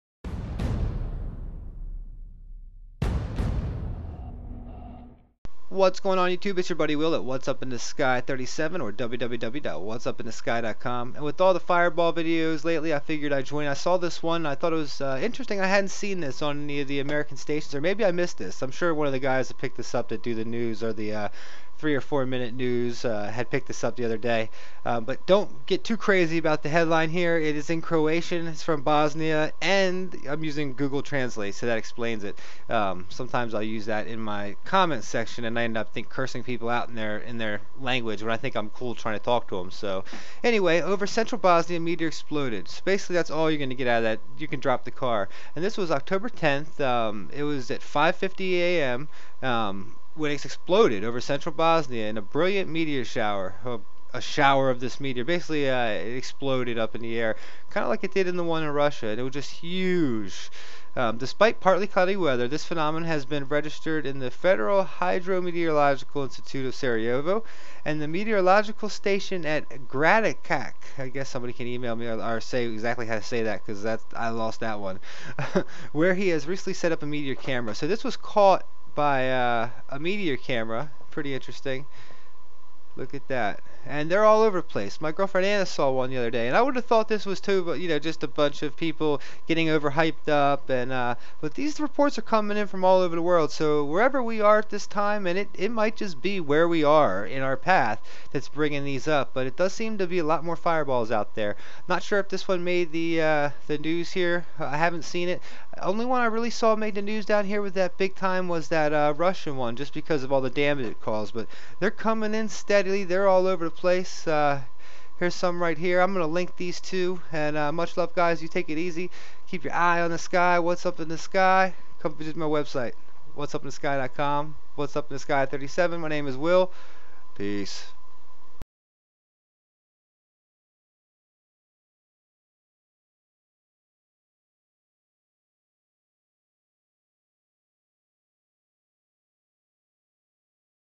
Meteor Explodes Over Bosnia @ 5:50 AM 10/10/2013
The fireballs have been gorgeous!  Many are seeing them int he night sky for their first times!  Good for you all!  No worries here just excited to see the shows! I am not sure if any of the feeds watching the upticks in activity around the world.  Just some more signs pointing to an amazing time coming for us!  If you have not seen one of these in person they are very awe inspiring and can really make you question what true beauty is!  These can also be disastrous as we saw over Russia..